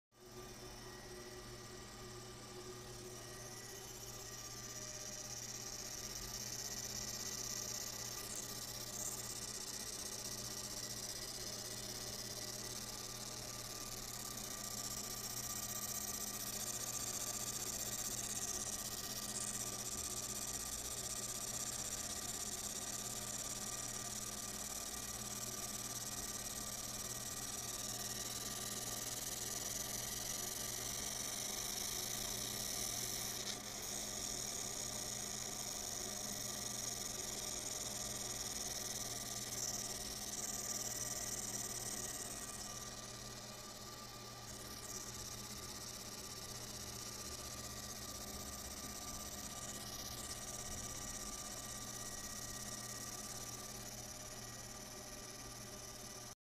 metal deep engraving by 50w fiber laser marking machine
50w fiber laser marking machine do deep engraving on stainless steel.